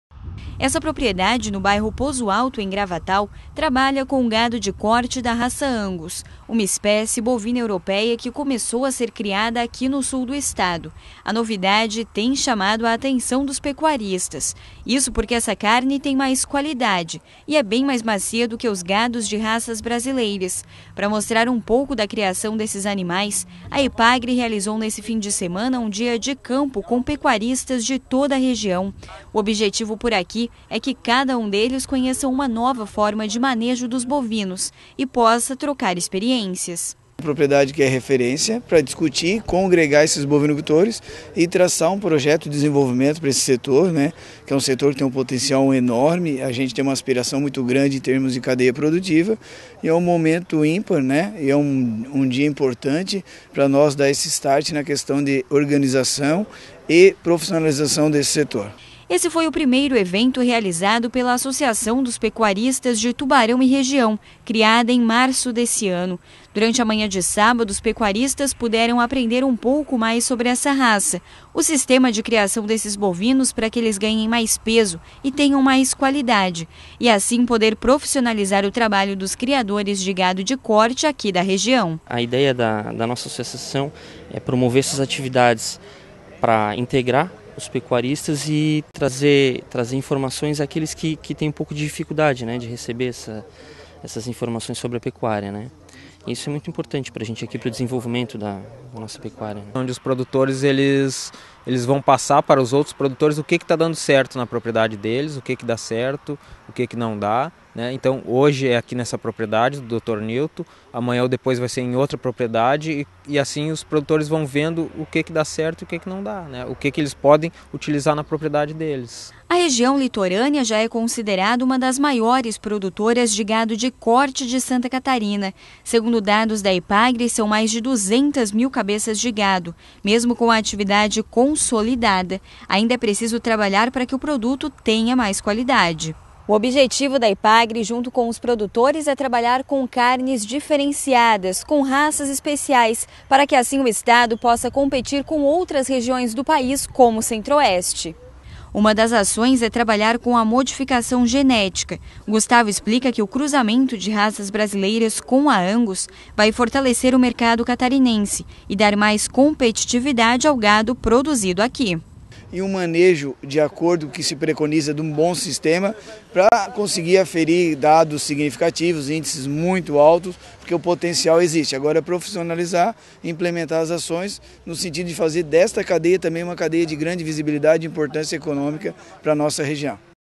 Epagri realiza Dia de Campo com Pecuaristas
A produção de gado de corte tem crescido na região litorânea, mas para que a Bovinocultura continue se desenvolvendo é preciso sempre renovar as formas de manejo e melhorar a qualidade do produto. Por isso, a Epagri realizou neste sábado, 24/6, um Dia de Campo com Pecuaristas da região. A intenção é promover uma troca de experiências entre os produtores